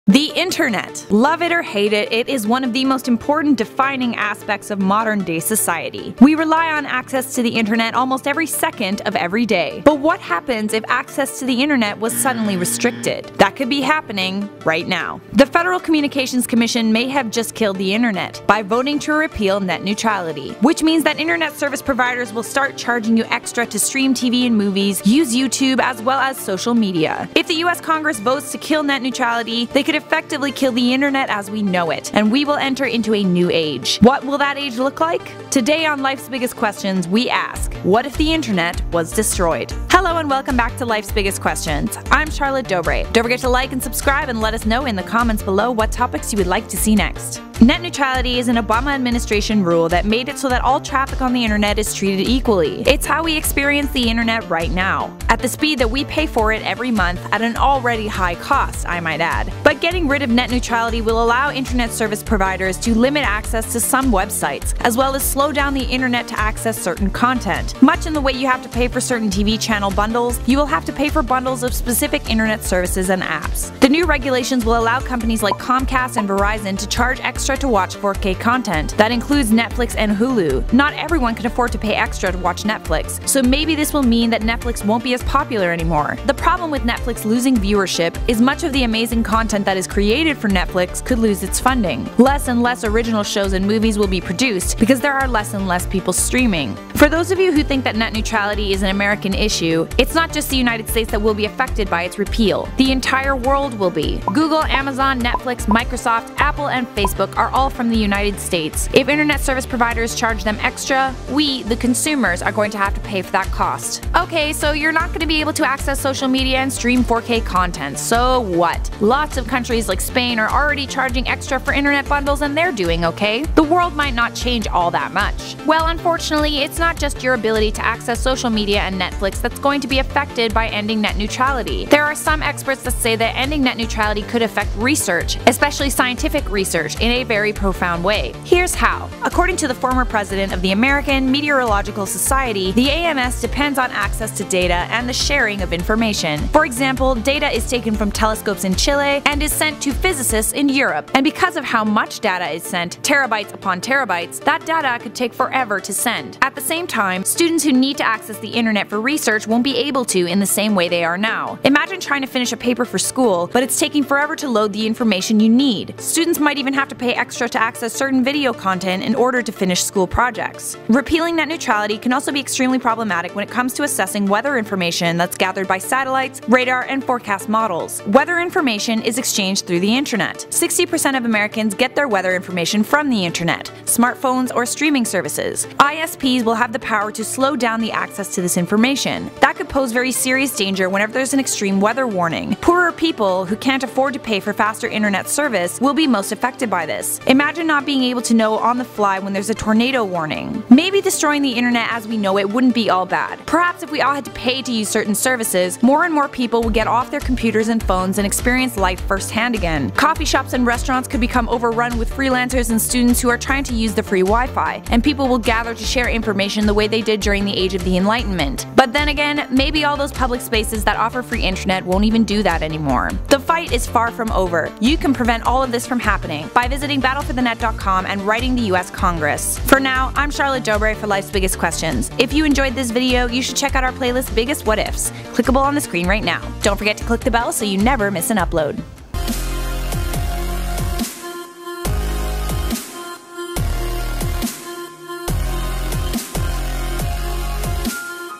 What If The Internet Was Destroyed – Net Neutrality
The internet. Love it or hate it, it is one of the most important defining aspects of modern day society. We rely on access to the internet almost every second of the day. But what happens, if access to the internet was suddenly restricted? That could be happening, right now. The Federal Communications commission may have just killed the internet, by voting to repeal net neutrality. Which means that internet service providers will start charging you extra to stream tv and movies, use youtube as well as social media. If the US congress votes to, kill net neutrality, they could effectively kill the internet as we know it, and we will enter into a new age. What will that age look like? Today, lifes biggest questions asks, what if the internet was destroyed?

VIDEO EDITED BY:
Imo Scrimger